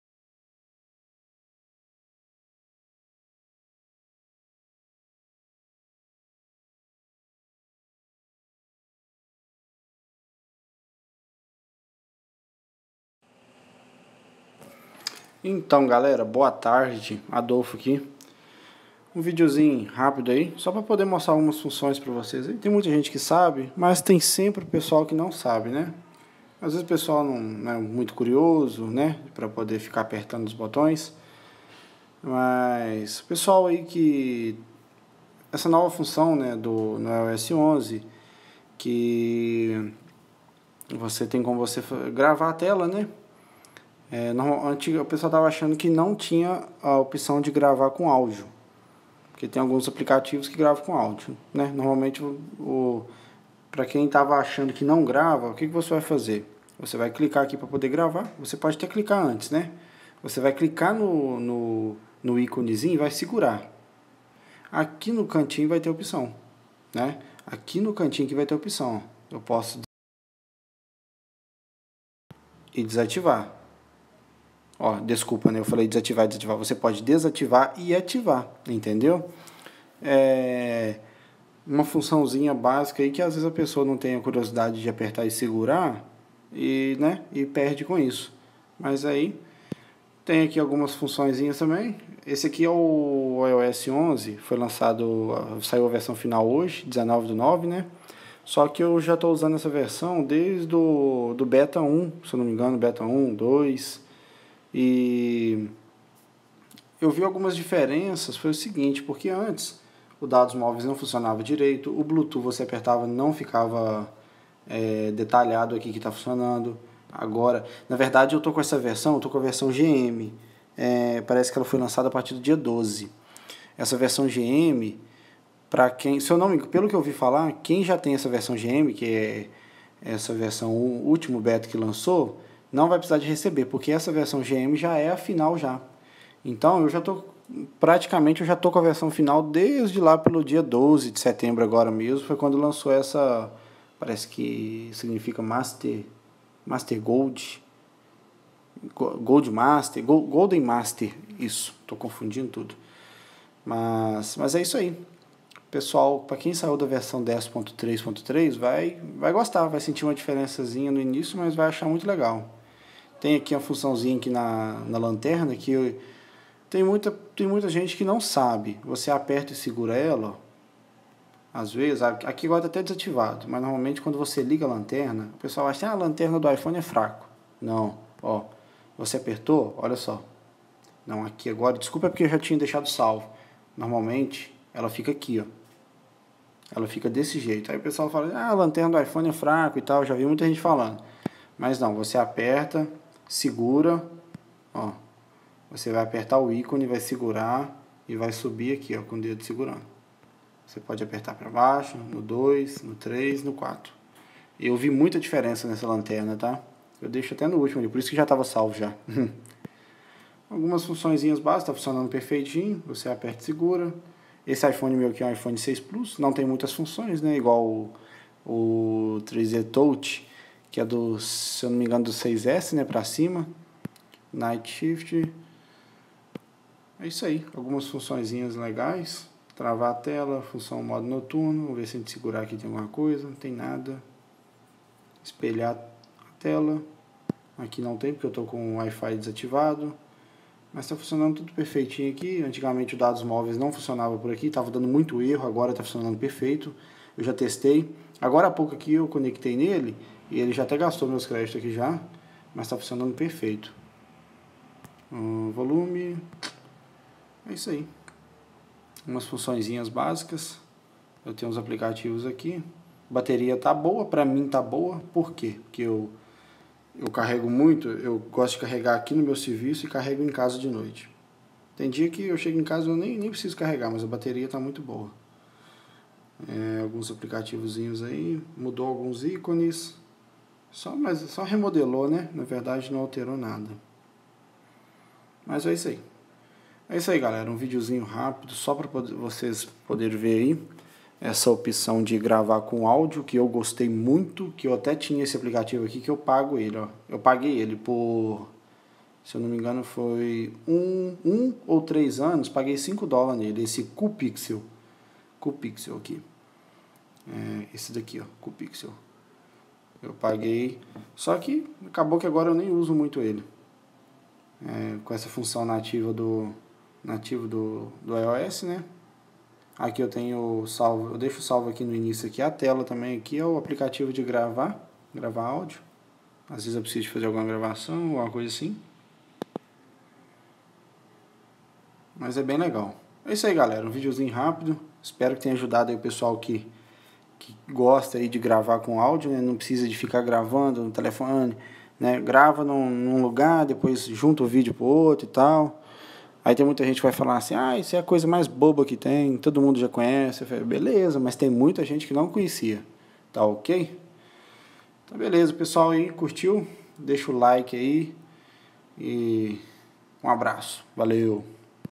iOS 11 FINAL e GRAVAR TELA DO IPHONE COM ÁUDIO
Pequena dica para a galera, e algumas pequenas funções no iOS 11!!